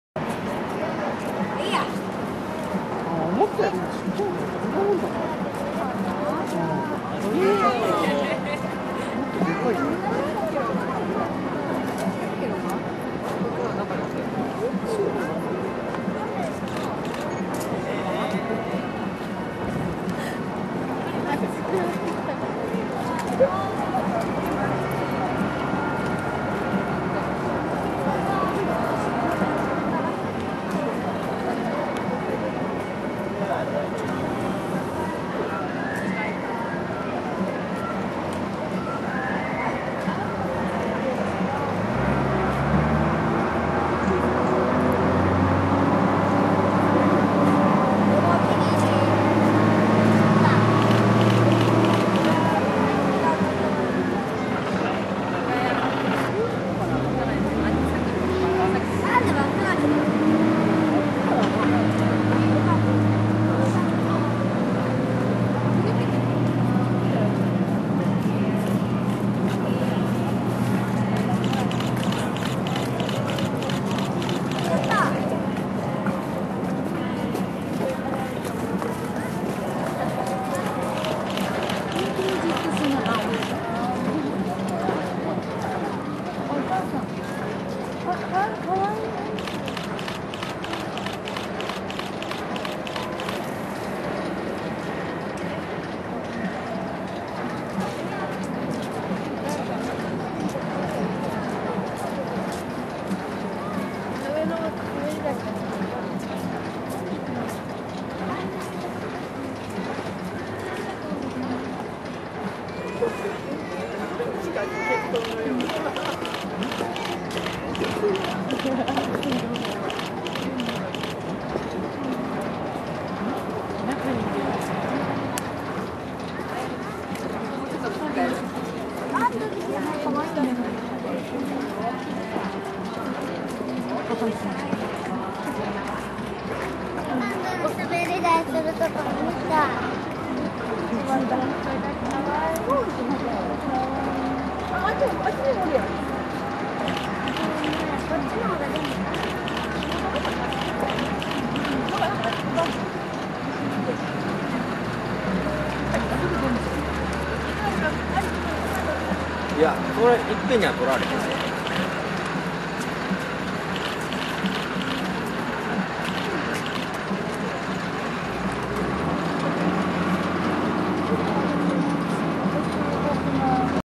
Giant Panda Twin Cubs Kaihin and Youhin of 2011 December at Wakayama, Japan #1-07
Youhin, Kaihin , twin babies born in Japan, 陽浜＆海浜 白浜 アドベンチャーワールド Youhin and Kaihin were 16-month old then.